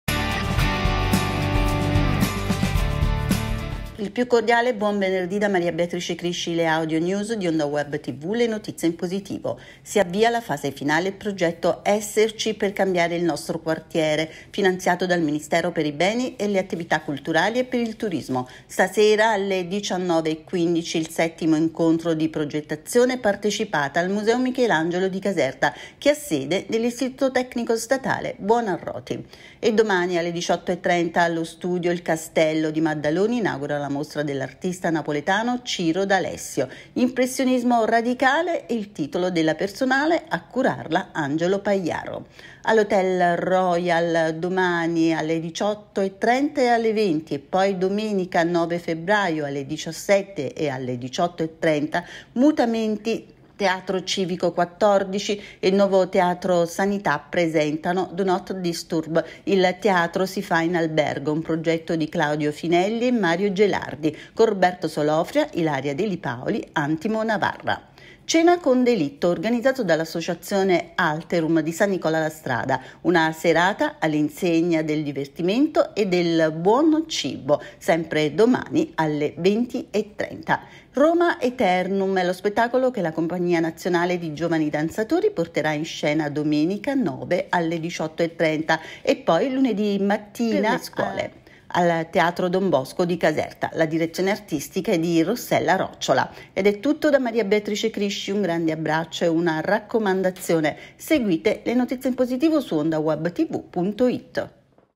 Audionews di Ondawebtv del 7 febbraio
Gli appuntamenti delle Audionews di Ondawebtv
*Museo Michelangelo. Oggi il progetto del Mibact sul quartiere
*Il Castello. A Maddaloni la personale di Ciro D'Alessio
*Hotel Royal. Il teatro si fa in albergo Do Not Disturb
*Alterum. Cena con delitto
*Roma-Aeternum in scena al teatro Don Bosco